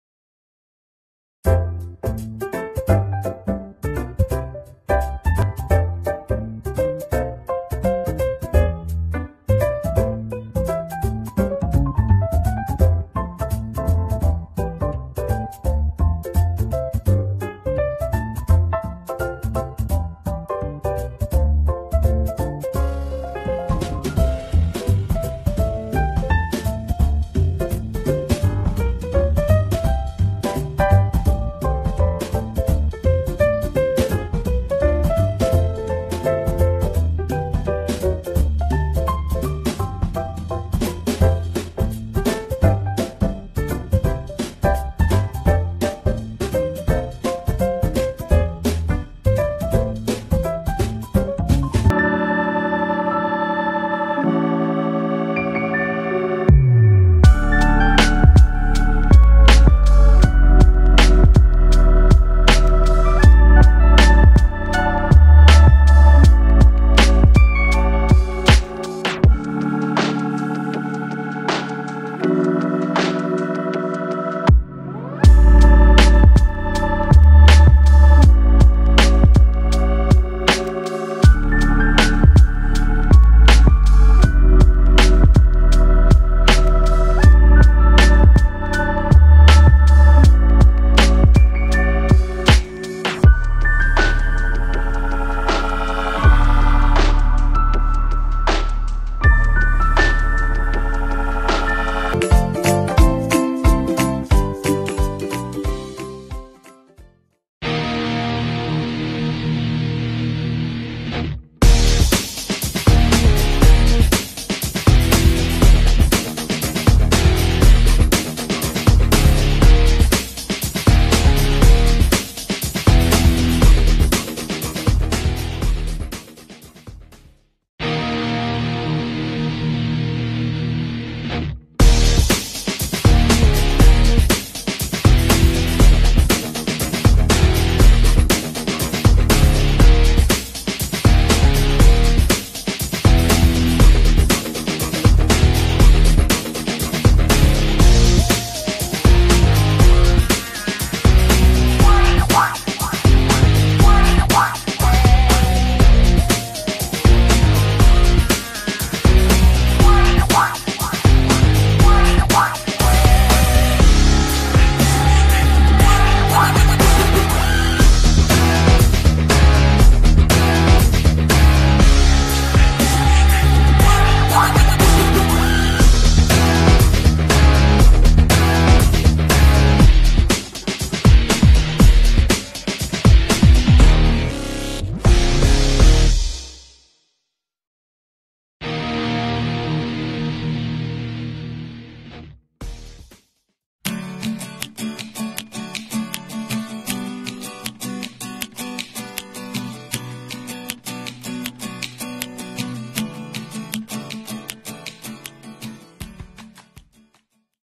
PJ masks Cake 🥰
Hello everyone. DIY PJ masks cake plus cake topper  for my baby boy.  Second birthday nya po. Happy Birthday DG boy.